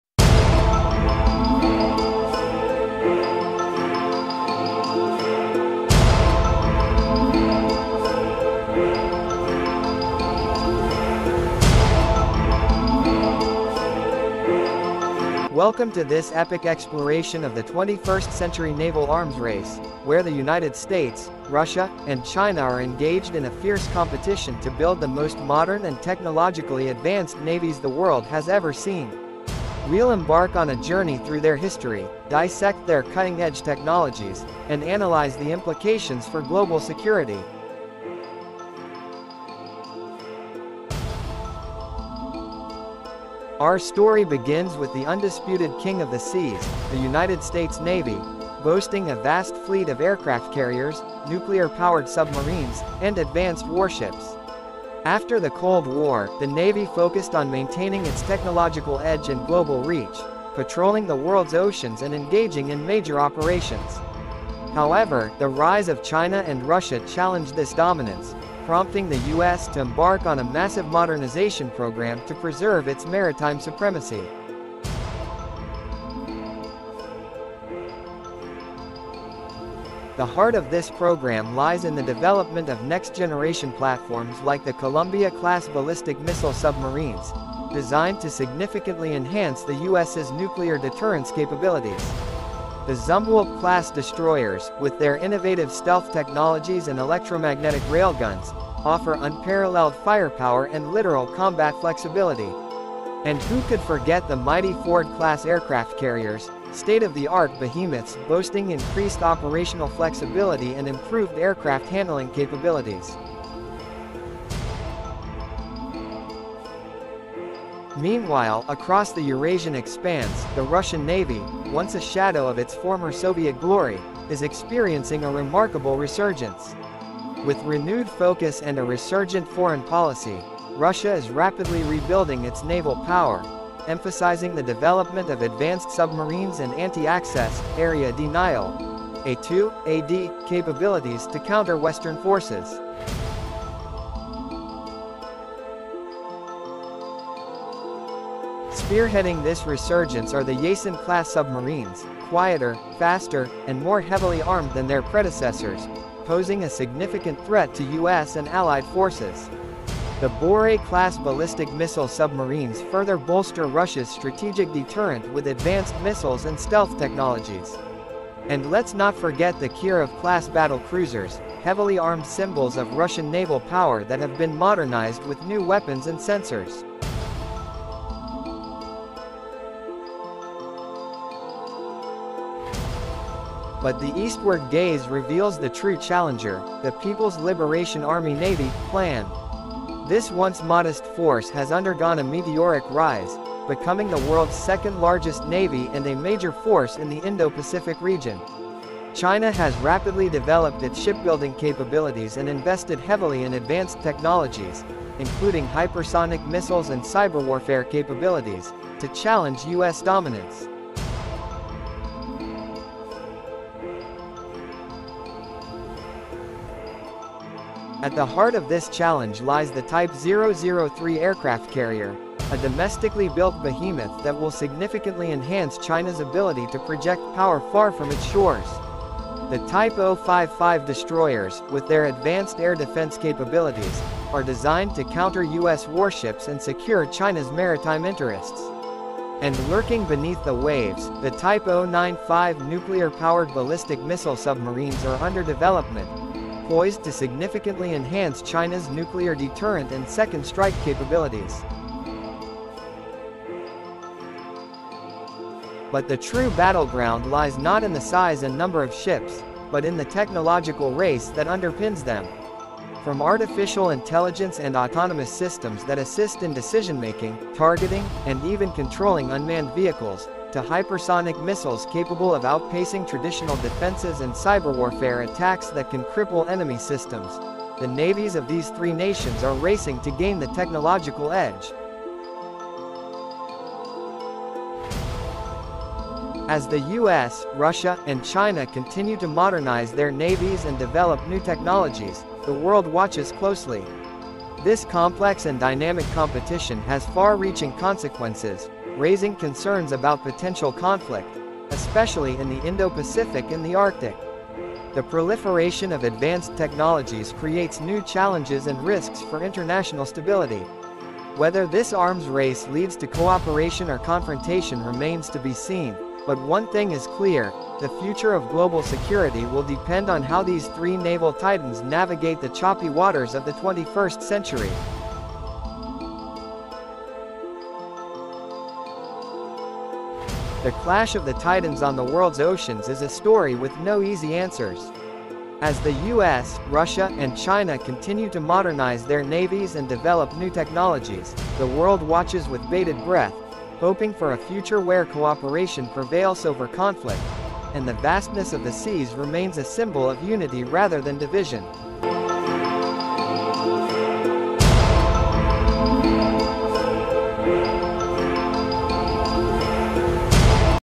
Naval Powerhouses Unveiled: 🇺🇸🇷🇺🇨🇳 | Clash of the Titans at Sea!
Clash of the Titans! US, Russia & China in a NAVAL ARMS RACE! 🇺🇸🇷🇺🇨🇳 Dive deep into the cutting-edge tech & modernization of these superpowers' navies. From AI-powered battleships to hypersonic missiles, this is a race you don't want to miss!
Click to watch & prepare to be AMAZED!